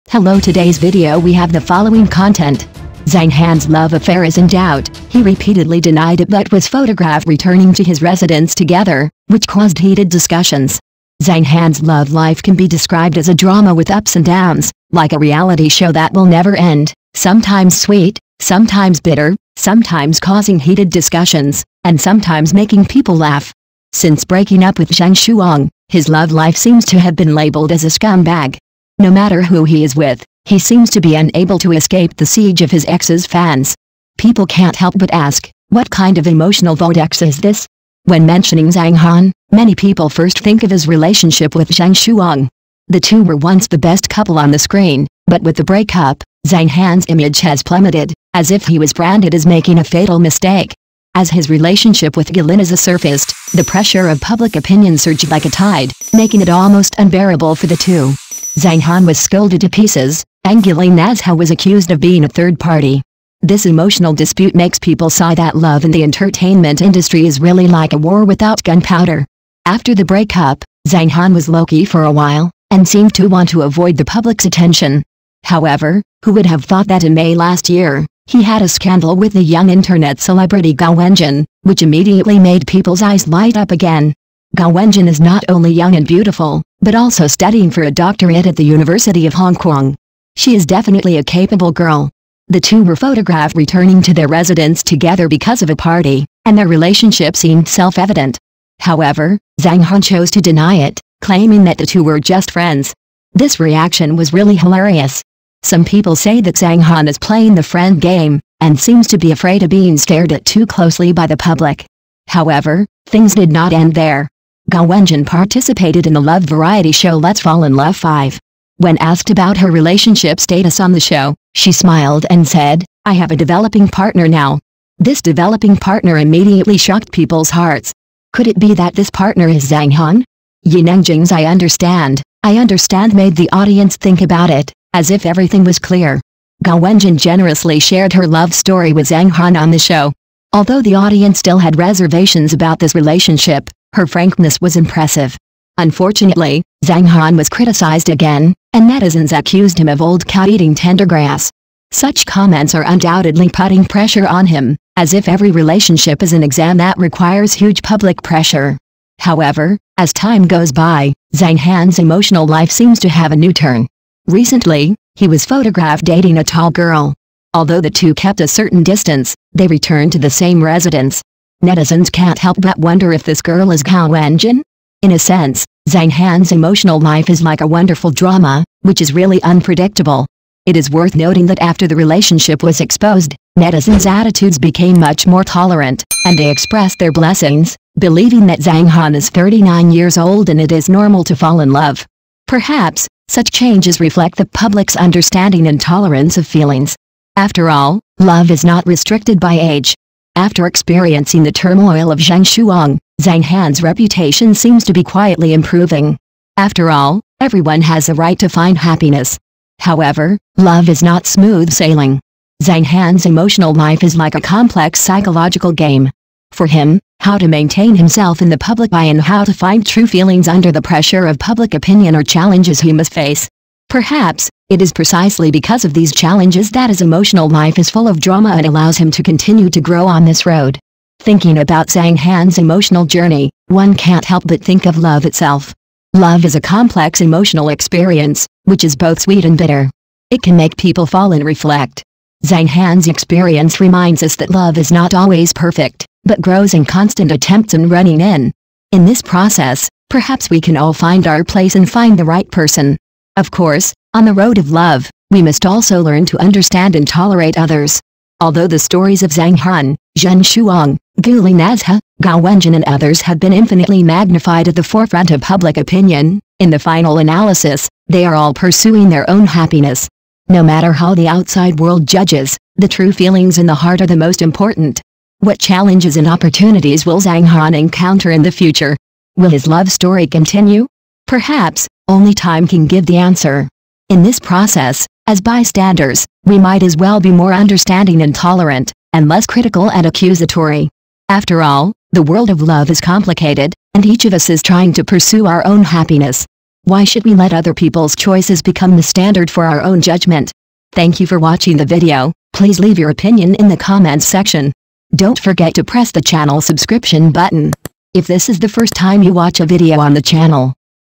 Zhang Han's love affair is in doubt: he repeatedly denied it but was photographed returning
Zhang Han's love affair is in doubt: he repeatedly denied it but was photographed returning to his residence together, which caused heated discussions
Zhang Han's love life has been full of twists and turns. After breaking up with Zheng Shuang, he was repeatedly criticized by the public. After his love affairs with Guli Nazha and Gao Wenjun were exposed, he was accused of "old cow eating young grass". Recently, his new love affair was exposed, and netizens were tolerant. Zhang Han's emotional journey is complicated, reminding that love requires trial and adjustment, and calling on the public to understand and tolerate.